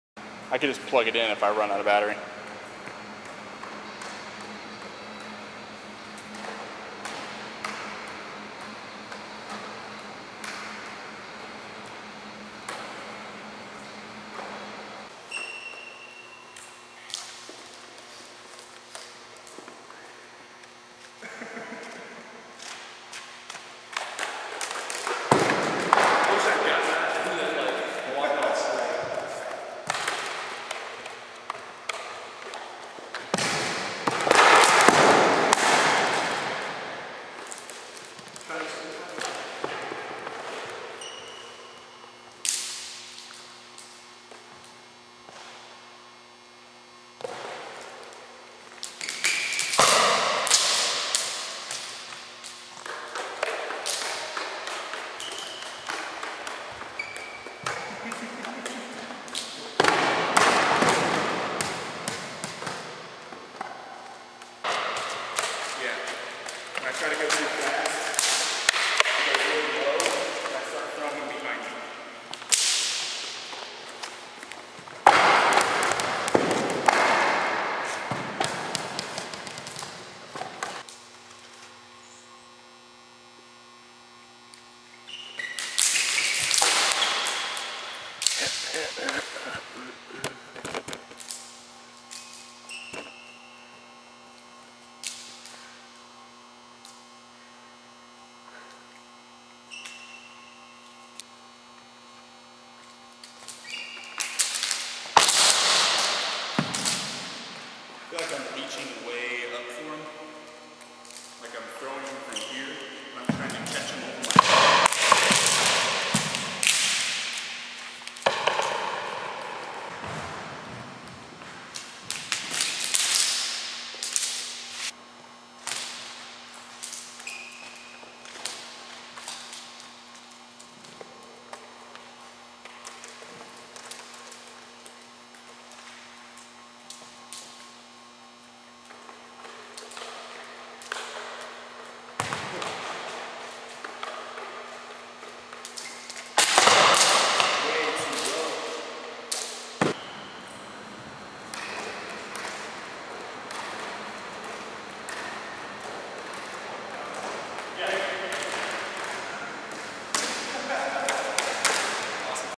Trip to NWA
Just a little video that I made from footage taken during my recent trip to Fayetteville.

The other jugglers are;
Eric Jackson
Jared Davis
Colby Leding